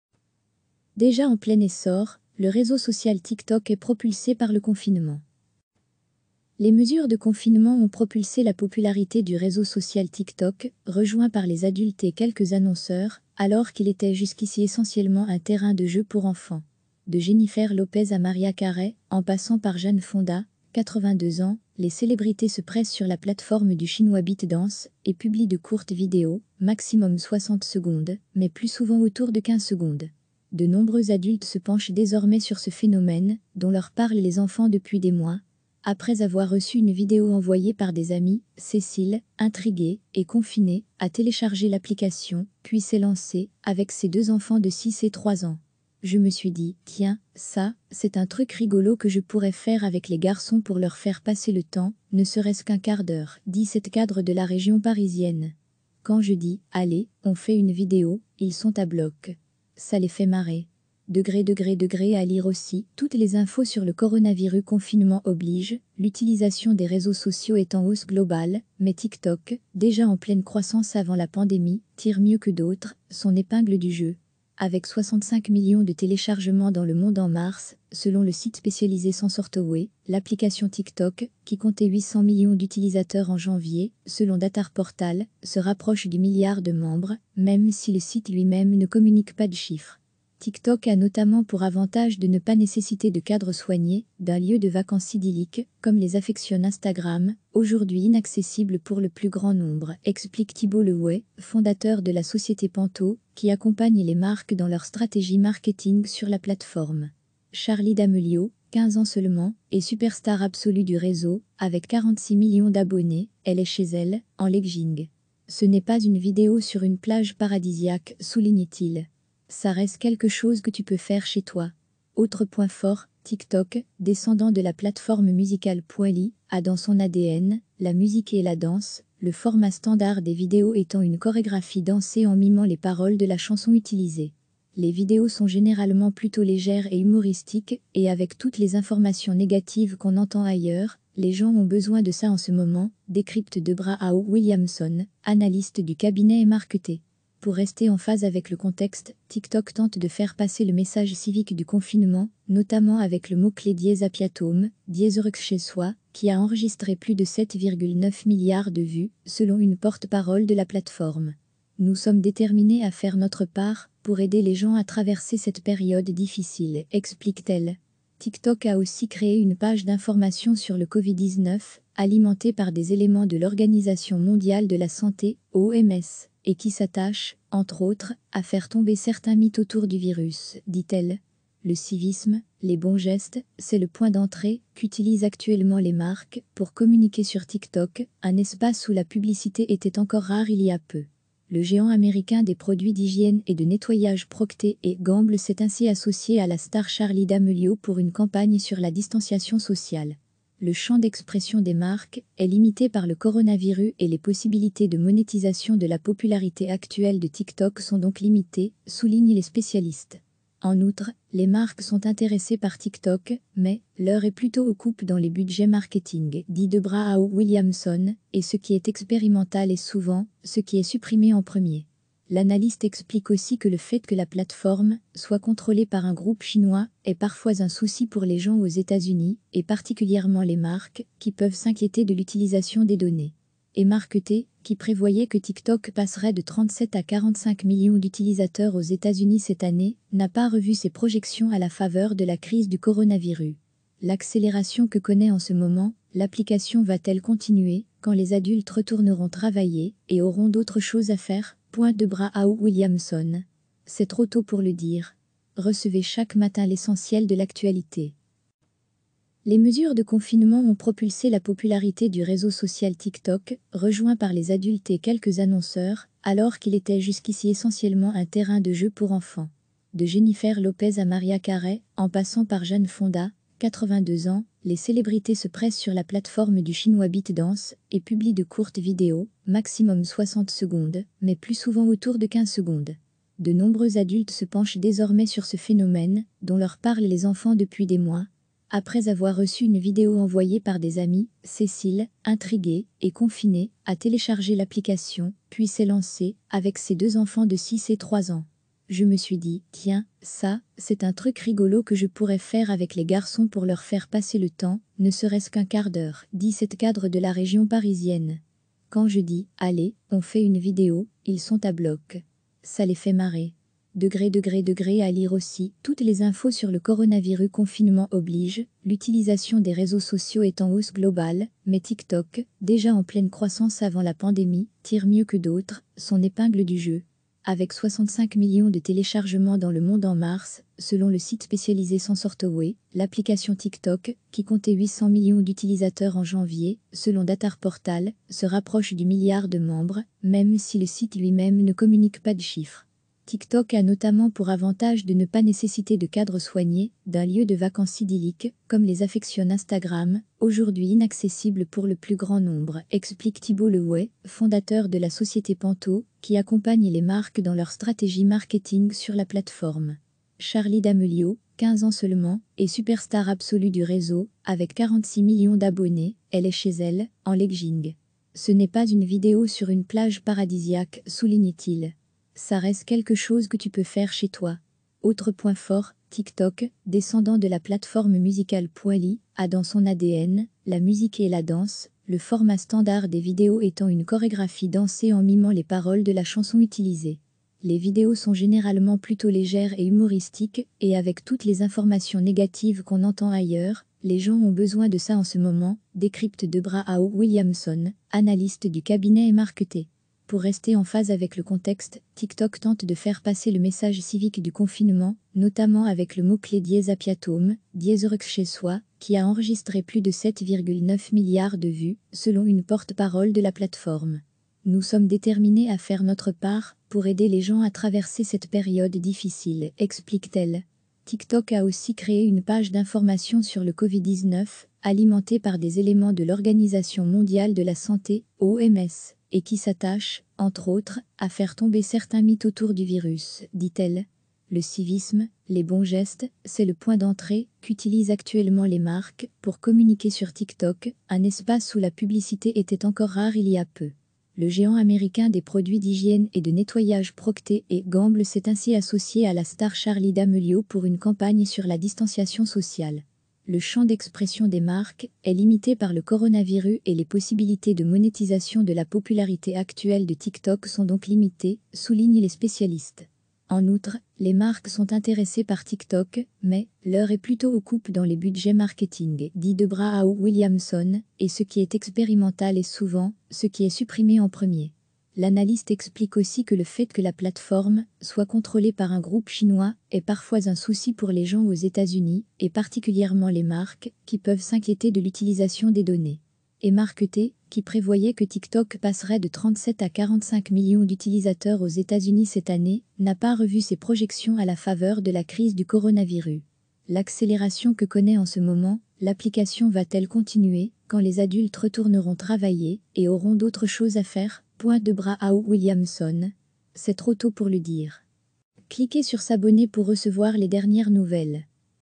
Déjà en plein essor, le réseau social TikTok est propulsé par le confinement
Les mesures de confinement ont propulsé la popularité du réseau social TikTok, rejoint par les adultes et quelques annonceurs alors qu’il était jusqu’ici essentiellement un terrain de jeu pour enfants.

De Jennifer Lopez à Mariah Carey, en passant par Jane Fonda, 82 ans, les célébrités se pressent sur la plateforme du Chinois ByteDance et publient de courtes vidéos (maximum 60 secondes, mais plus souvent autour de 15 secondes).

De nombreux adultes se penchent désormais sur ce phénomène dont leur parlent les enfants depuis des mois.

Après avoir reçu une vidéo envoyée par des amis, Cécile, intriguée et confinée, a téléchargé l’application, puis s’est lancée, avec ses deux enfants de 6 et 3 ans.

"Je me suis dit : tiens, ça, c’est un truc rigolo que je pourrais faire avec les garçons pour leur faire passer le temps, ne serait-ce qu’un quart d’heure", dit cette cadre de la région parisienne.

"Quand je dis : allez, on fait une vidéo, ils sont à bloc. Ça les fait marrer."

►►► À lire aussi : Toutes les infos sur le coronavirus

Avec 65 millions de téléchargements dans le monde en mars, selon le site spécialisé SensorTower, l’application TikTok, qui comptait 800 millions d’utilisateurs en janvier (selon DataReportal), se rapproche du milliard de membres, même si le site lui-même ne communique pas de chiffres.

TikTok a notamment pour avantage de ne pas nécessiter de cadre soigné, d’un lieu de vacances idyllique, comme les affectionne Instagram, aujourd’hui inaccessibles pour le plus grand nombre, explique Thibault Le Ouay, fondateur de la société Pentos, qui accompagne les marques dans leur stratégie marketing sur la plateforme.

"Charli D’Amelio", 15 ans seulement et superstar absolue du réseau avec 46 millions d’abonnés, "elle est chez elle, en leggings. Ce n’est pas une vidéo sur une plage paradisiaque", souligne-t-il. "Ça reste quelque chose que tu peux faire chez toi."

Autre point fort, TikTok, descendant de la plateforme Musical.ly, a dans son ADN la musique et la danse, le format standard des vidéos étant une chorégraphie dansée en mimant les paroles de la chanson utilisée.

"Les vidéos sont généralement plutôt légères et humoristiques, et avec toutes les informations négatives qu’on entend ailleurs, les gens ont besoin de ça en ce moment", décrypte Debra Aho Williamson, analyste du cabinet eMarketer.

Pour rester en phase avec le contexte, TikTok tente de faire passer le message civique du confinement, notamment avec le mot-clé happyathome (heureuxchezsoi), qui a enregistré plus de 7,9 milliards de vues, selon une porte-parole de la plateforme.

"Nous sommes déterminés à faire notre part pour aider les gens à traverser cette période difficile", explique-t-elle.

TikTok a aussi créé une page d’information sur le Covid-19, alimentée par des éléments de l’Organisation mondiale de la santé (OMS) et qui s’attache, entre autres, à "faire tomber certains mythes" autour du virus, dit-elle.

Le civisme, les bons gestes, c’est le point d’entrée qu’utilisent actuellement les marques pour communiquer sur TikTok, un espace où la publicité était encore rare il y a peu.

Le géant américain des produits d’hygiène et de nettoyage Procter & Gamble s’est ainsi associé à la star Charli D’Amelio pour une campagne sur la distanciation sociale.

Le champ d’expression des marques est limité par le coronavirus et les possibilités de monétisation de la popularité actuelle de TikTok sont donc limitées, soulignent les spécialistes.

En outre, "les marques sont intéressées par TikTok mais […] l’heure est plutôt aux coupes dans les budgets marketing", dit Debra Aho Williamson, "et ce qui est expérimental est souvent ce qui est supprimé en premier".

L’analyste explique aussi que le fait que la plateforme soit contrôlée par un groupe chinois est parfois "un souci pour les gens aux Etats-Unis, et particulièrement les marques", qui peuvent s’inquiéter de l’utilisation des données.

eMarketer, qui prévoyait que TikTok passerait de 37 à 45 millions d’utilisateurs aux Etats-Unis cette année, n’a pas revu ses projections à la faveur de la crise du coronavirus.

L’accélération que connaît en ce moment l’application "va-t-elle continuer quand les adultes retourneront travailler et auront d’autres choses à faire ?" pointe Debra Aho Williamson. "C’est trop tôt pour le dire".

De Jennifer Lopez à Mariah Carey, en passant par Jane Fonda, 82 ans, les célébrités se pressent sur la platefor